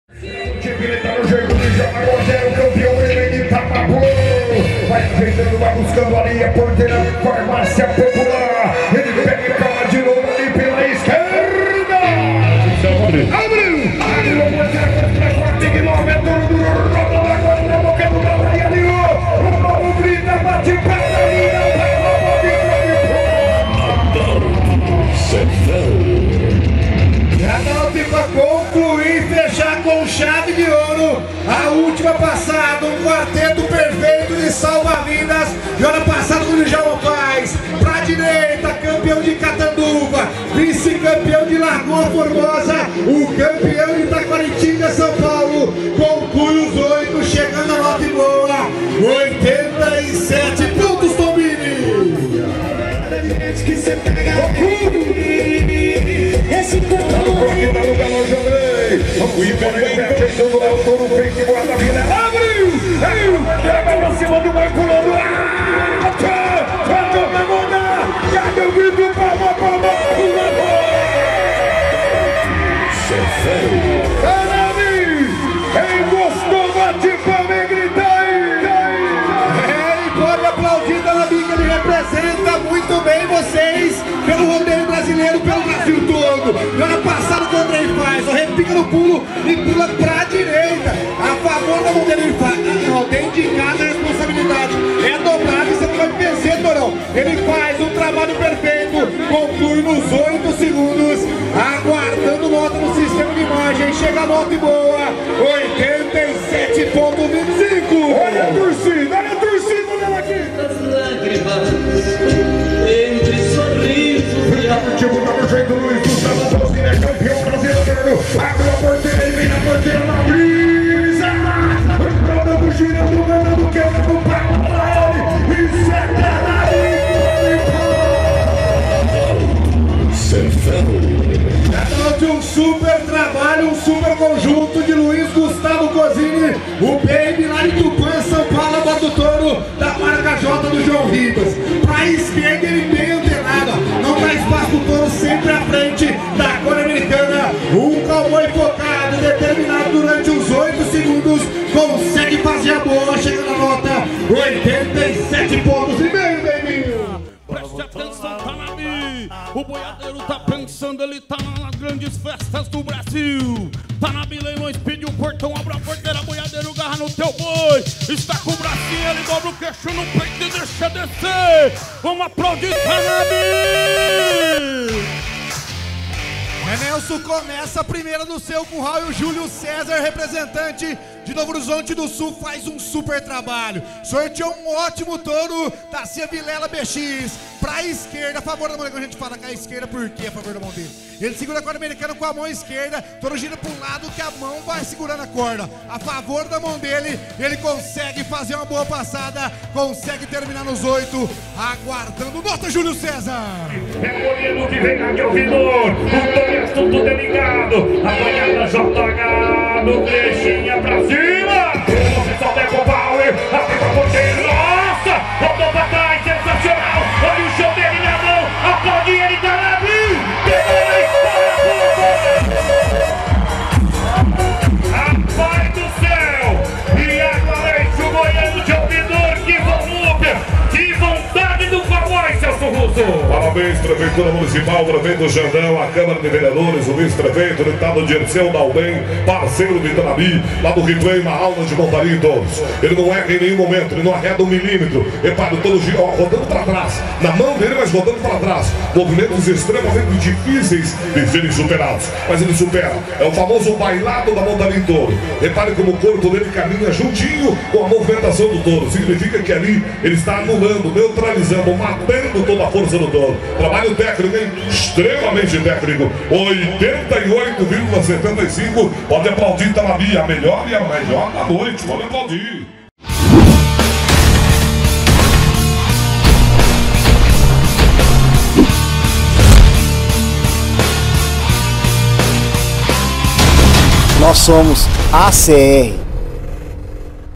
TANABI-SP 2023 | MELHORES MONTARIAS - QUINTA FEIRA | 37º ETAPA
37ª Etapa ACR BGB SUPER CUP | Temporada 2023

E a maior nota do 1º Round, foi do competidor IAGO ALEIXO ALVES de OUVIDOR-GO, após obter 88,75 pts, montando no Touro ASSUNTO DELICADO (Cia. JH/CRECHINHA)

Confira as Melhores Montarias

Imagens e Edições: HENRIQUE BRAZÃO